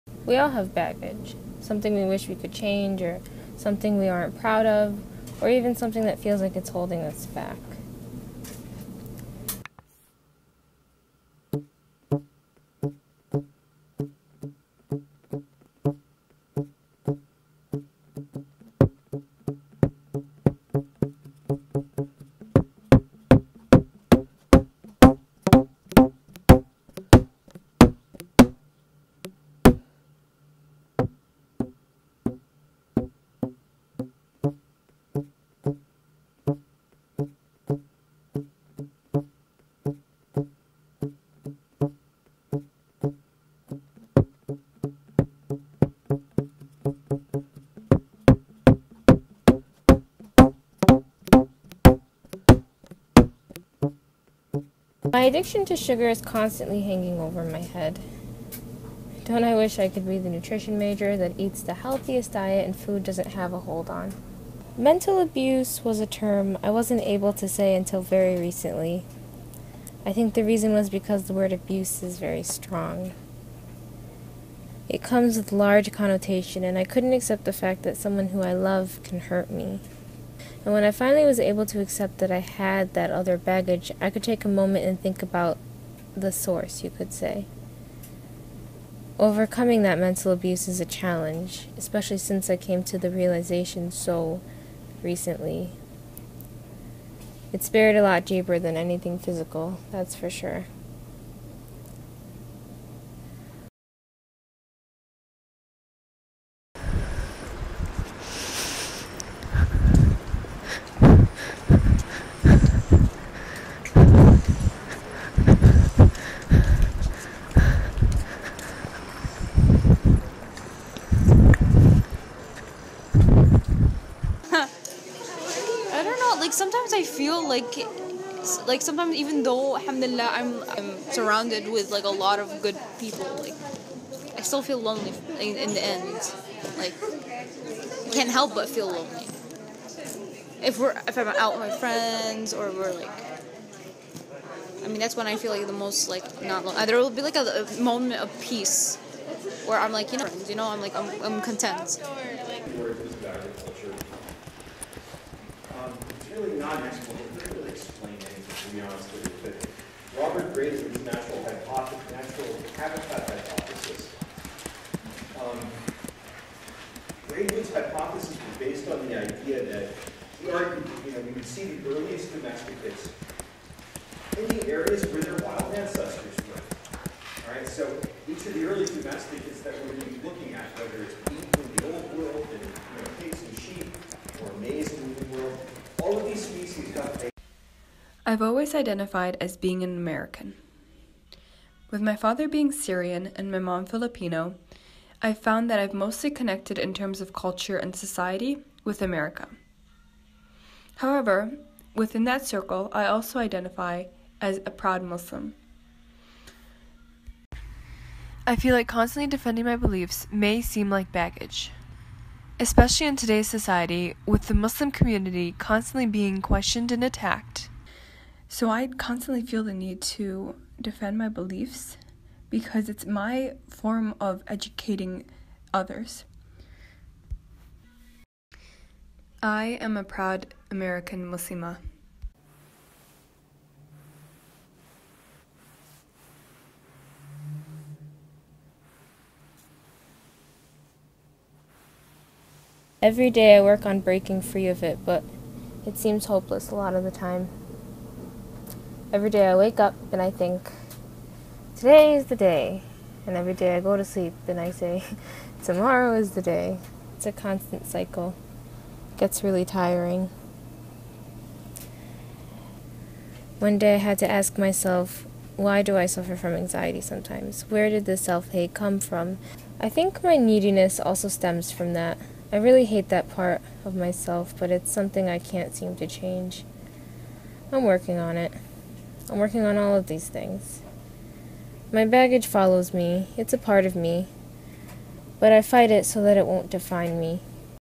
Our Baggage Short Film | SimplyNabzDesign
The following is a short film I created on the different ways in which people interact with internal difficulties. The blue suitcase symbolizes the baggage each of us carries in our lives.
Some of us run from our baggage, others find it haunting them, we give baggage to others through our interactions and sometimes we learn to either live with or find a way to let go of our baggage.

Special thanks to all the actors and actresses who helped bring this film alive by volunteering their time.

To be honest this isn't the greatest video quality wise. I made it as part of a class project while still new to filming. We were not allowed to use any sound files and had to create our own, hence the weird sound effects. It is what it is, and I still love the story.